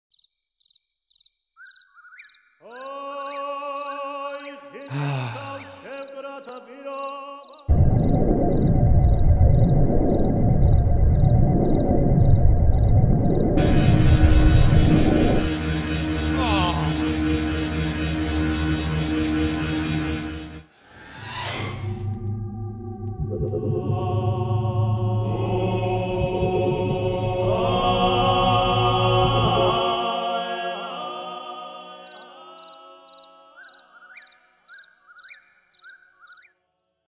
Abduction || Funny Stories by Studio BAZI
A short funny cartoon about alien abduction.

CREDIT
Artist - Alex Bronzoff &  Ihor Zheved